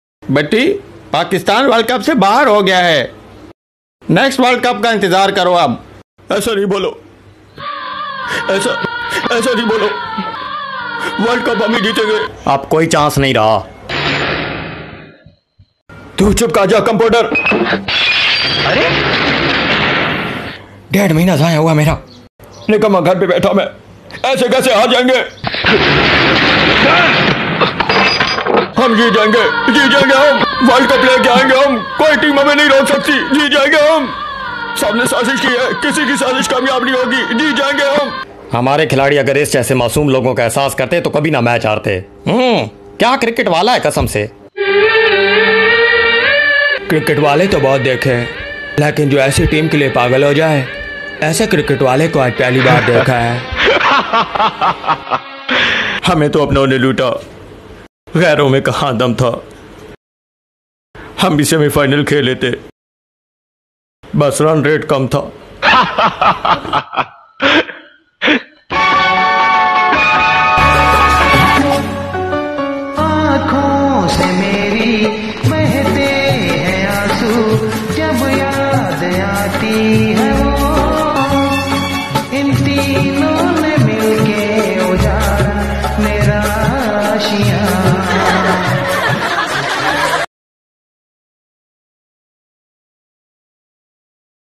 world cup comedy
Pakistan world cup ab Kabhi nhi jitega